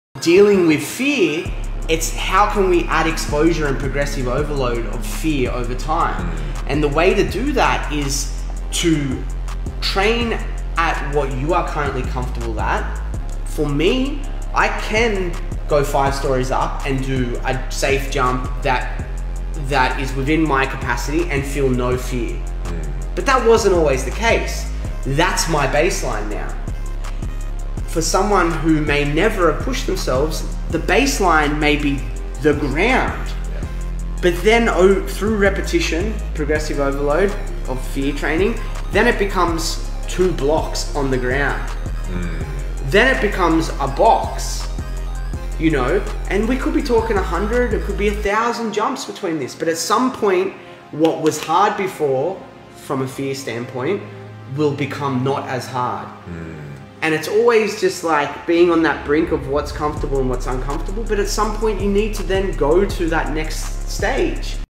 KILL FEAR
Overcoming fear is tough. In weight training we understand the concept of progressive overload. Can the same technique be used for mental training?